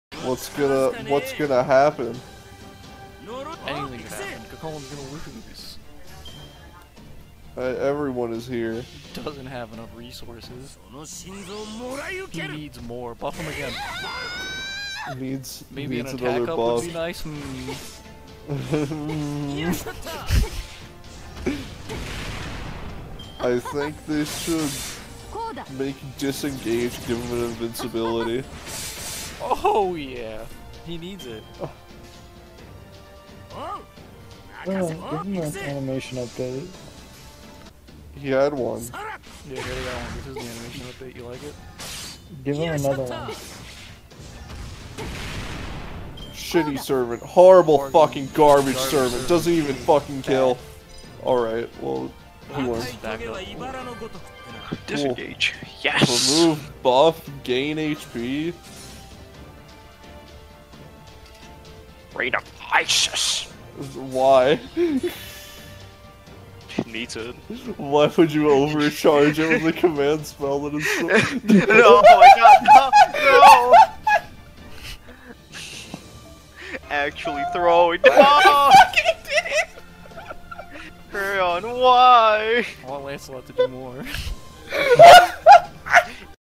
Cu Chulainn Kills Skadi
I wasn't content with winning via the iconic "Cu Safety Net™"
this is why I'm always missing command spells in my videos btw